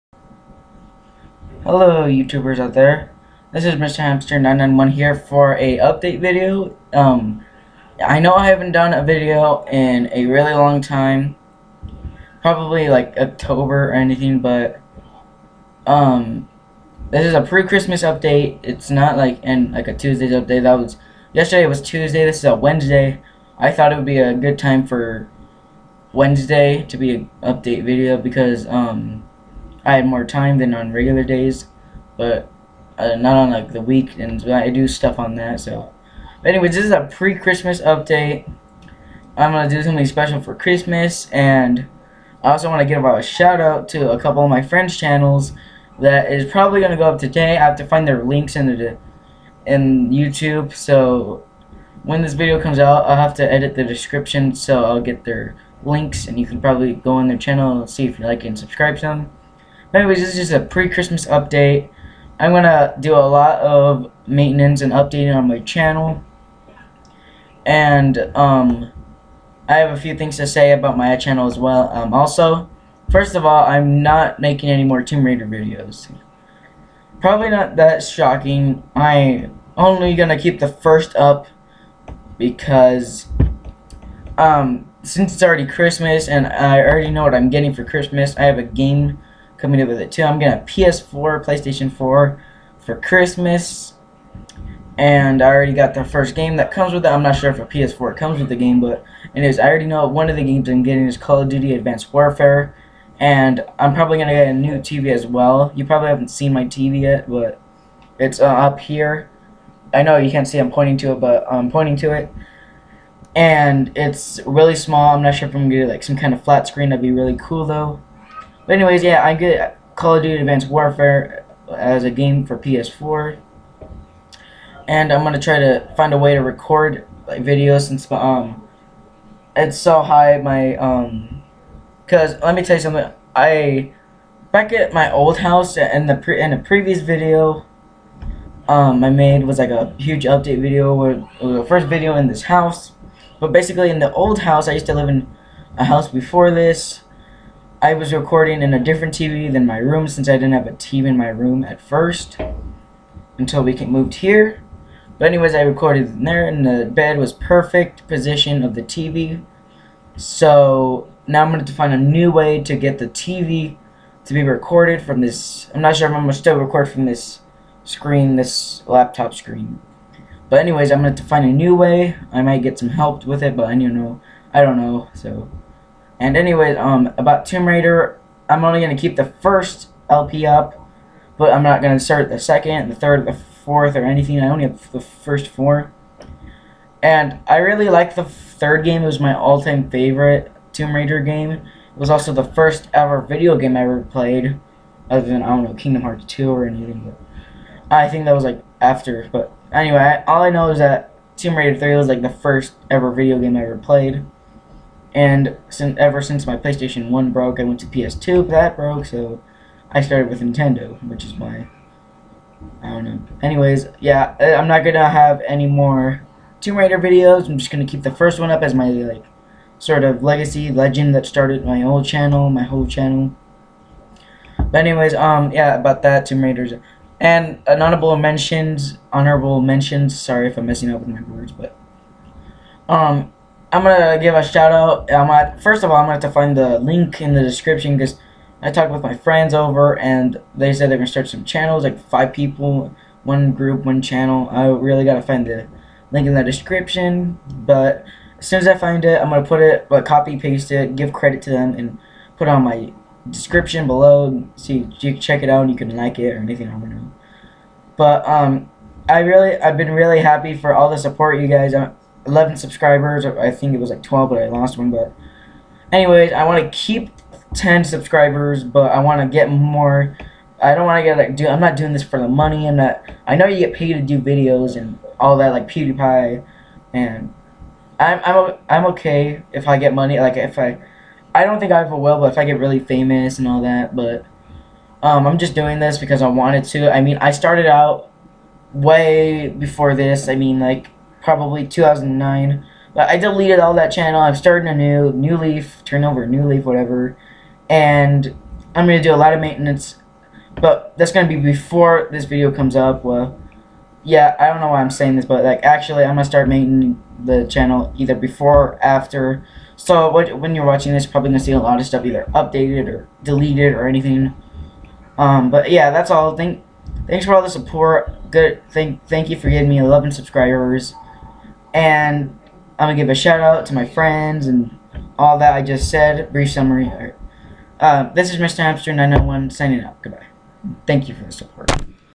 Pre-Christmas Update: No more Tomb Raider!
No more Tomb Raider! Also, this is a Pre-Christmas Update, meaning that there will be a video for Christmas.

I will take all the support I can get.

Thx for watching and enjoy!